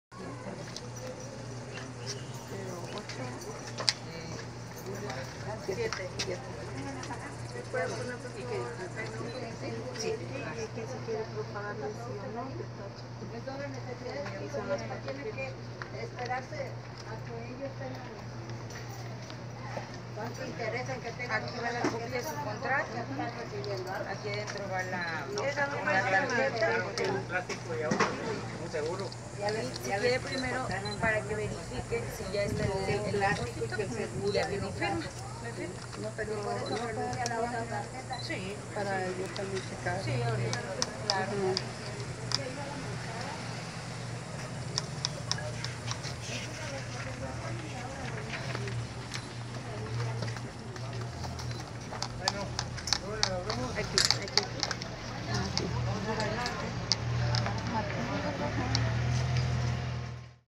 Pensionados de Salud se manifiestan en el Civil
Una manifestación más contra @TamaulipasSalud, entérate quién y qué pedían